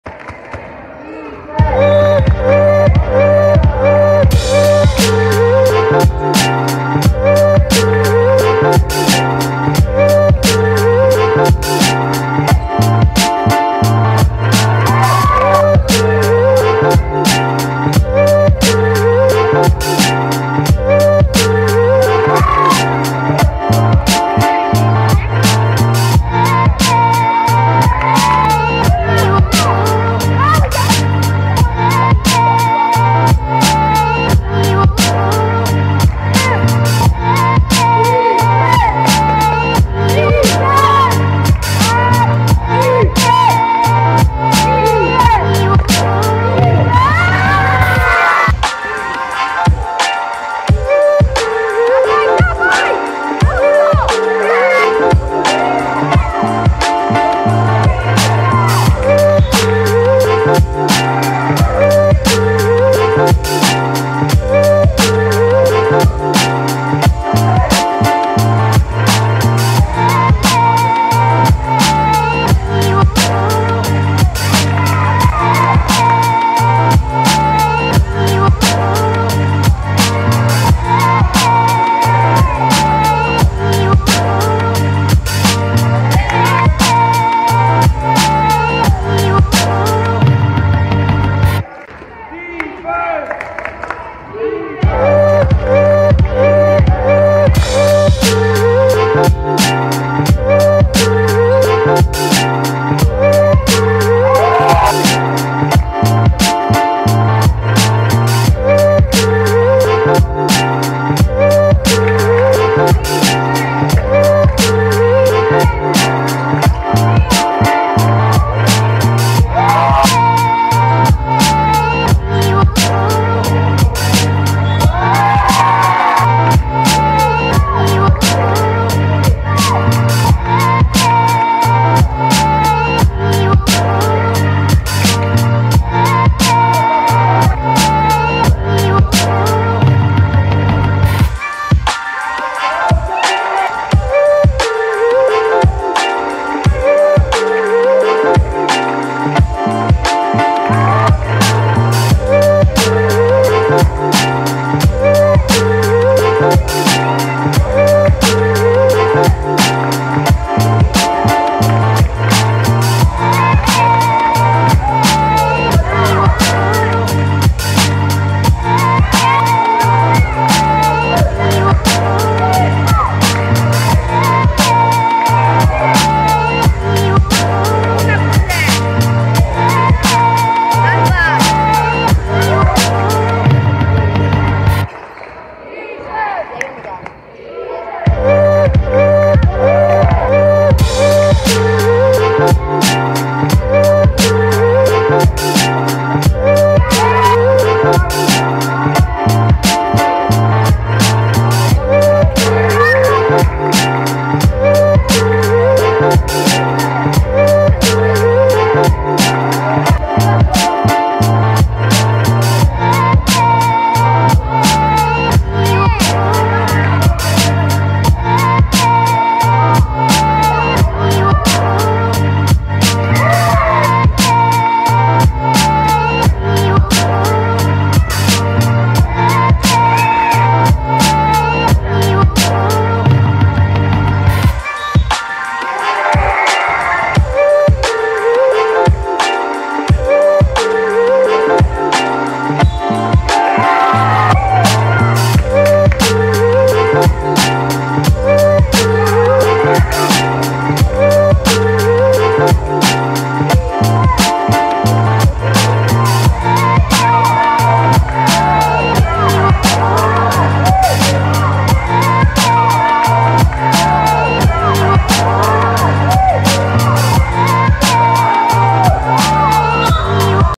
18 07 San Antonio vs Chicago – DC89 Junior Brodnica 2023
SAN ANTONIO 58:65 CHICAGO
J.Świdurski 15pkt 4x3 8zb
M.Ostrowski 18pkt 7zb 3ast